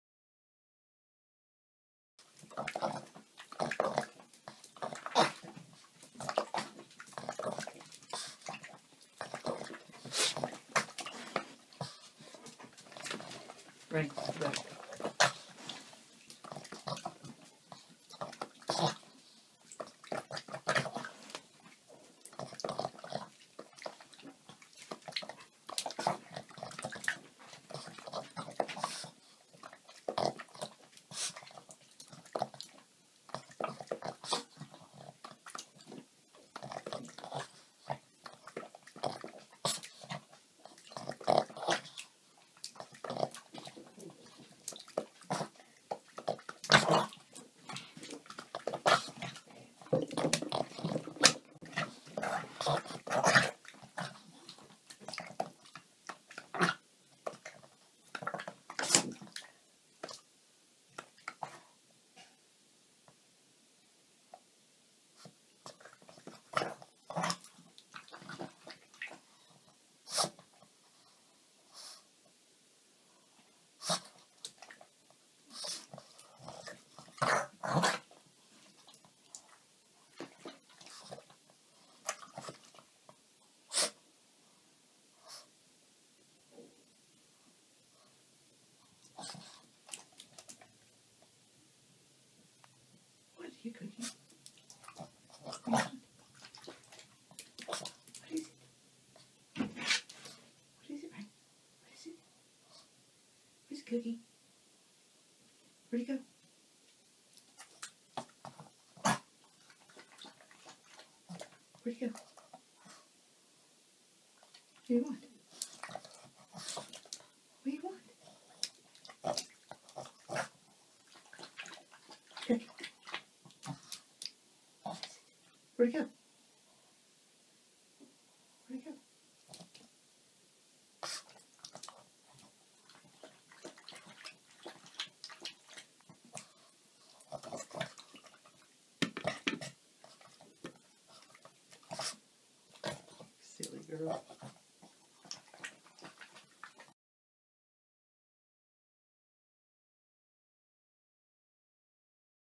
Cookie: My Silly Girl
She does this each and every time I go into the bathroom... jumps over in the tub and cries until I turn the water on... she has a blast biting at the water... but she is not happy when I turn it off...As you see... lol